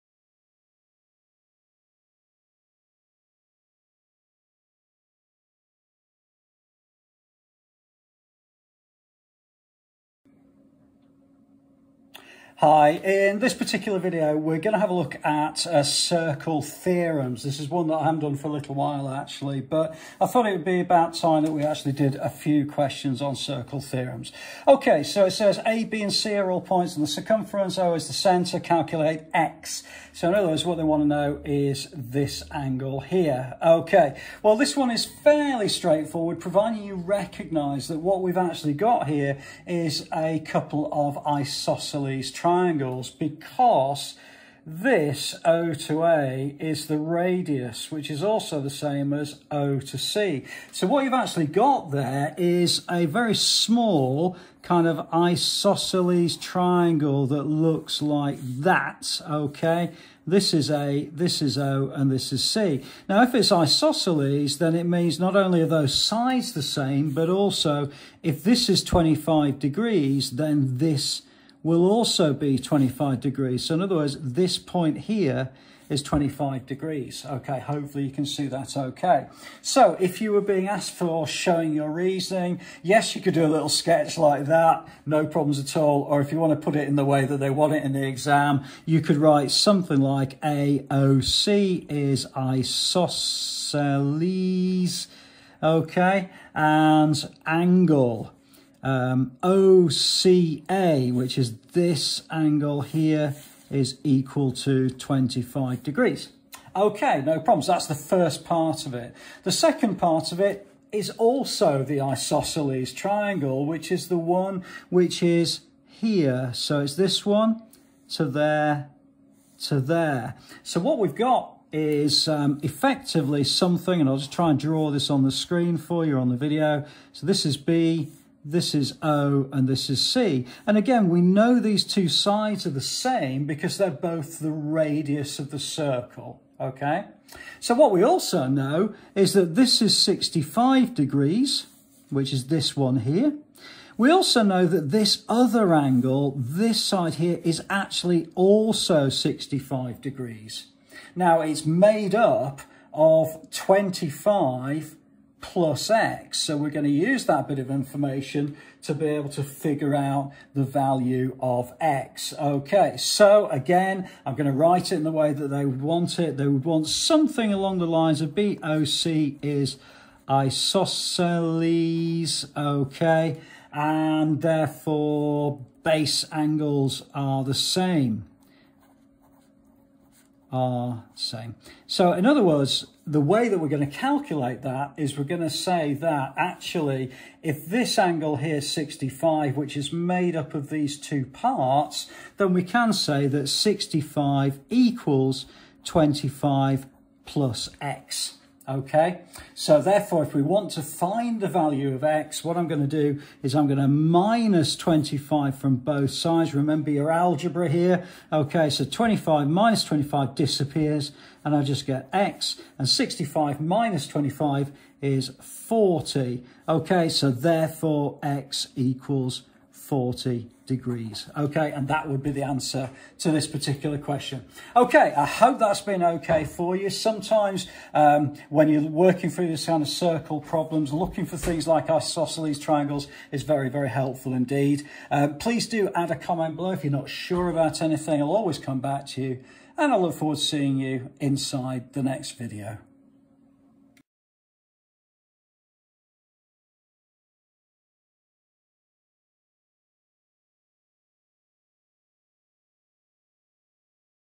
Circle Theorems - GCSE maths question 1
@3minutemaths for quick reminder and GCSE maths walkthrough videos.

This video is all about circle theorems and forms part of the playlist  of the same name.

The question is aimed at grade 7 GCSE maths:

A, B, and C are all points on the circumference of a circle.
O is the centre of the circle.
Calculate angle x, showing your reasoning.
Unfortunately I can't draw the diagram in the description, but I hope the thumbnail helps.